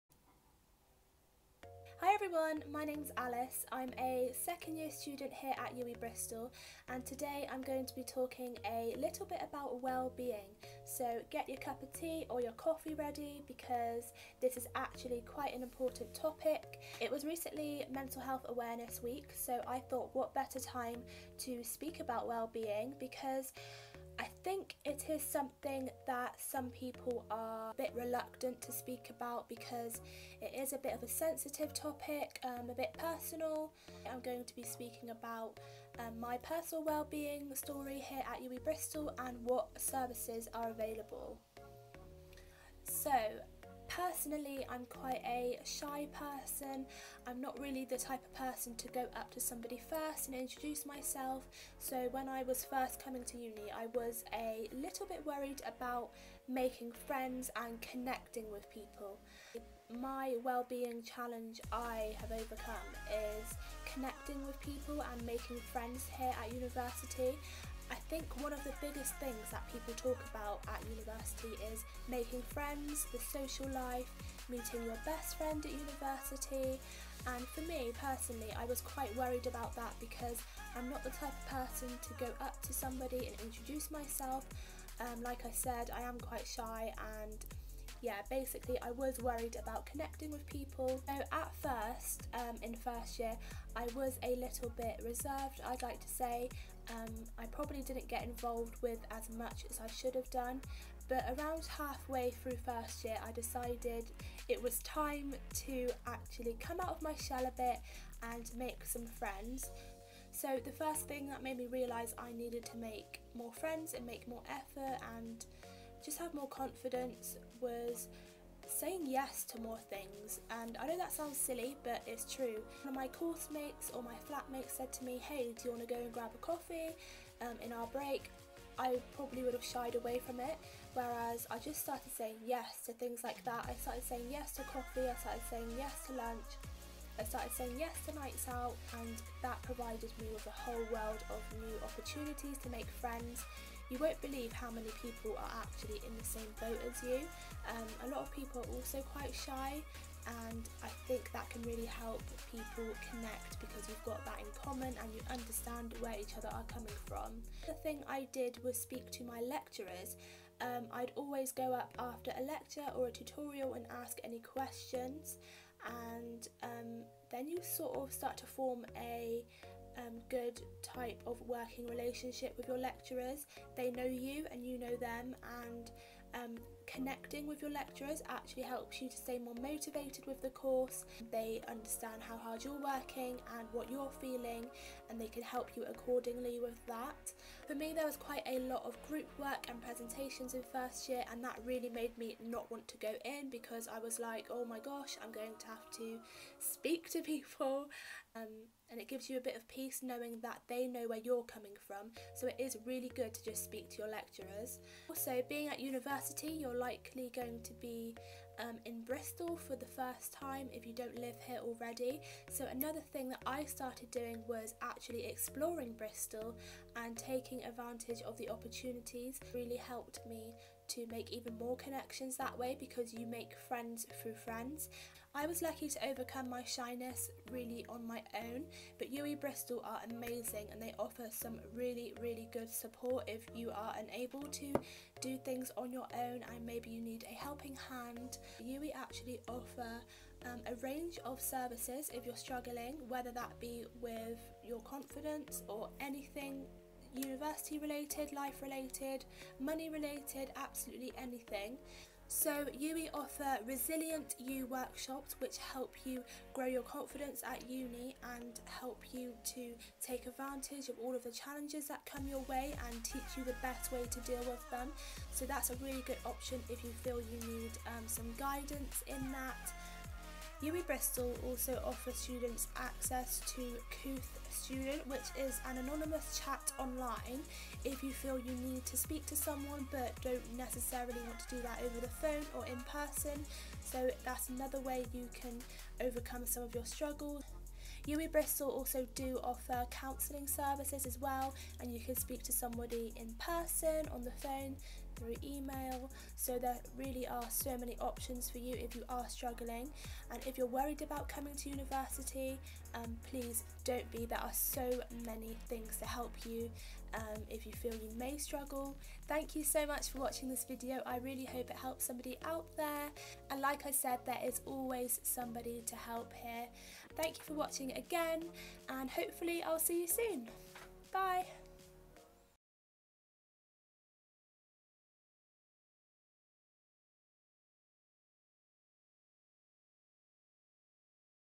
MyUWEBristol - Alice discusses wellbeing and support options
2nd year student Alice discusses her experience of the wellbeing and support options at UWE Bristol